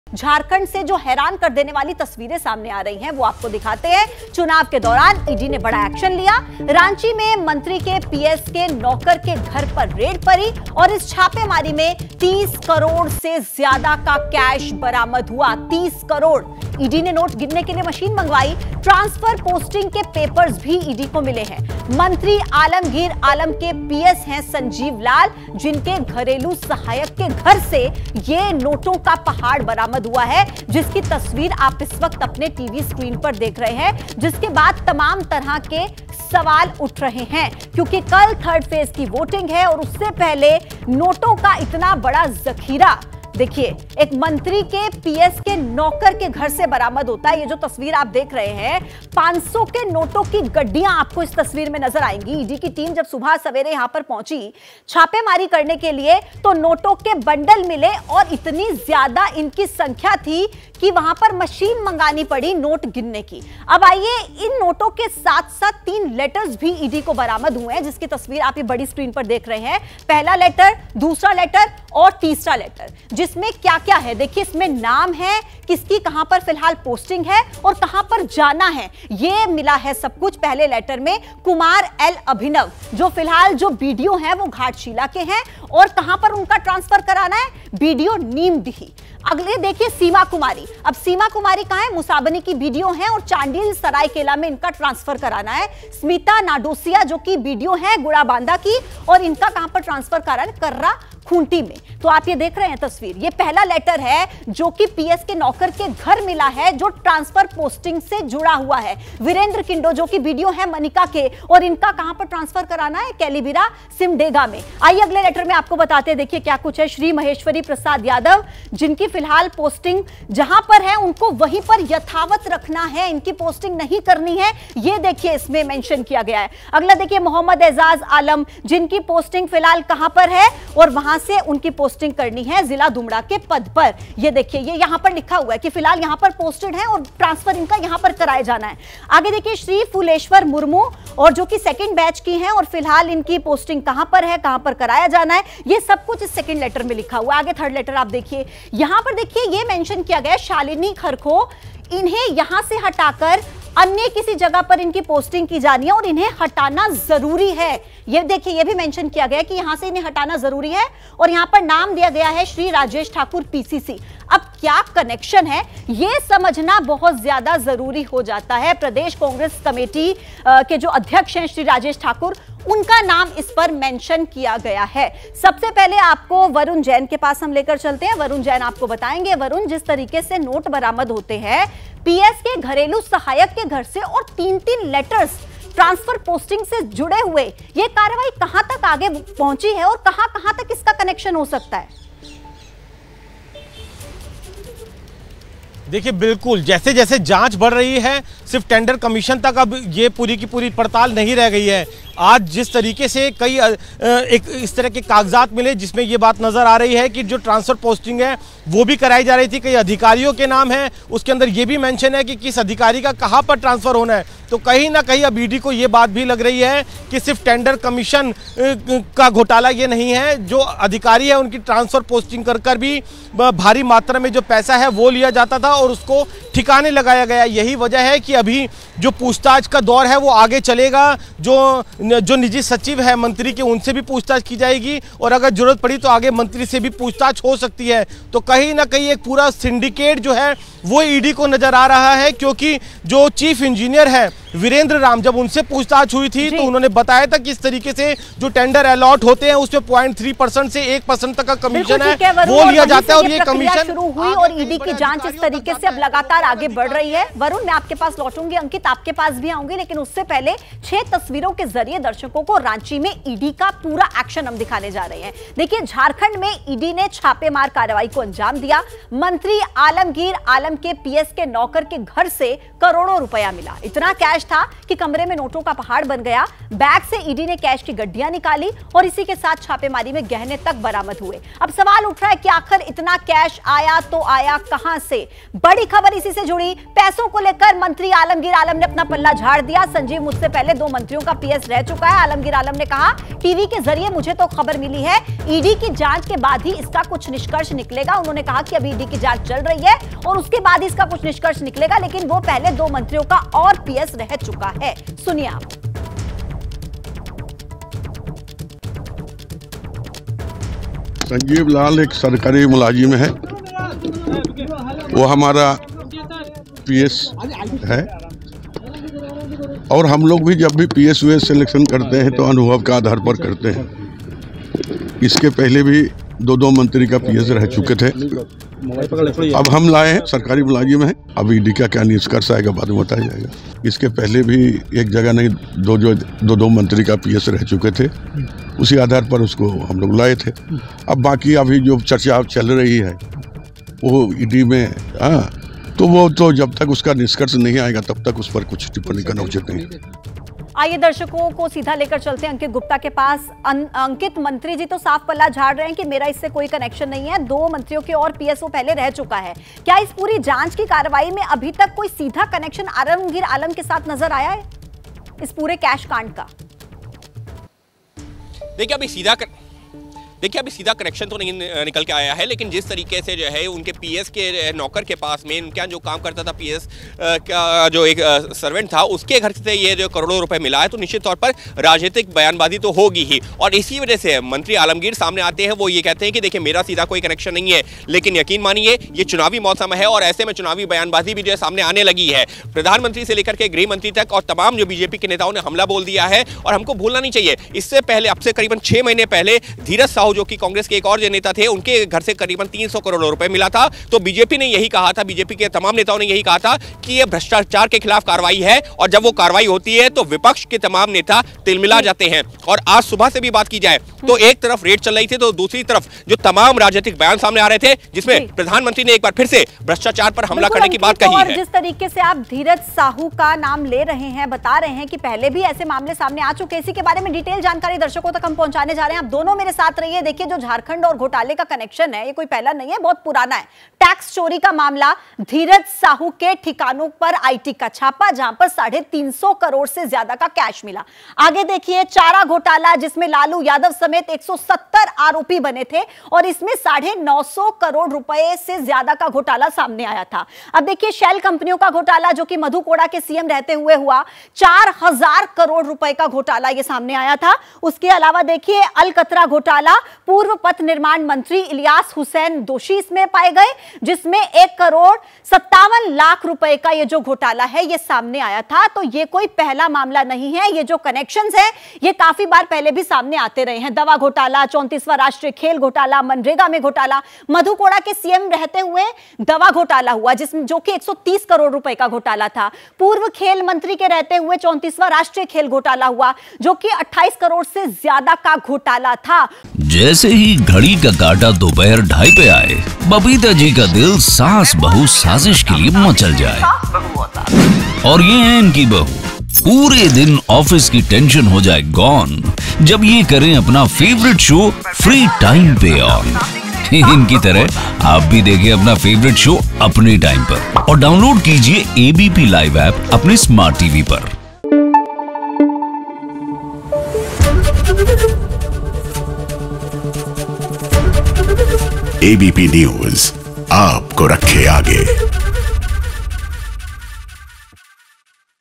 Breaking News: झारखंड से हैरान करने वाली तस्वीरें,चुनाव के दौरान ED का बड़ा एक्शन जारी | ED Raid
झारखंड से हैरान करने वाली तस्वीरें,चुनाव के दौरान ED का बड़ा एक्शन जारी,रांची में मंत्री के PS के नौकर के घर रेड जारी है..